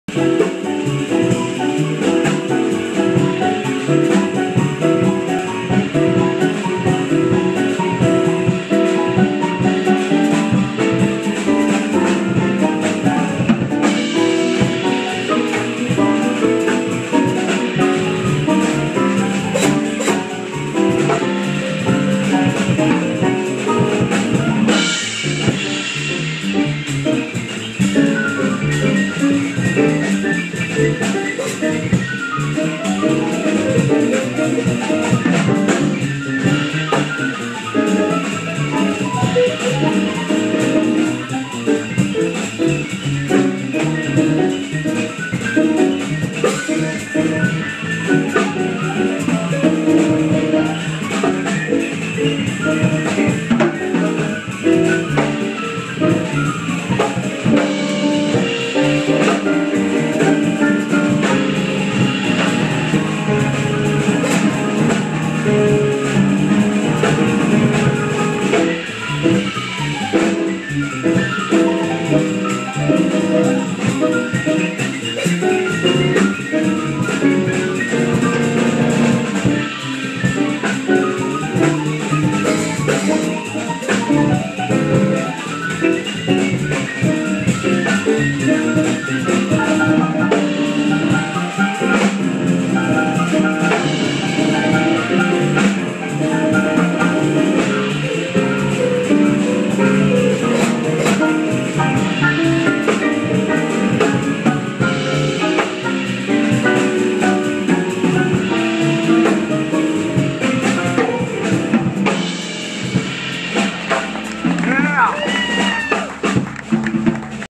Cyrus Chestnut solo ....
The great pianist Cyrus Chestnut playing with the Shares Cassity Quintet at Iridium.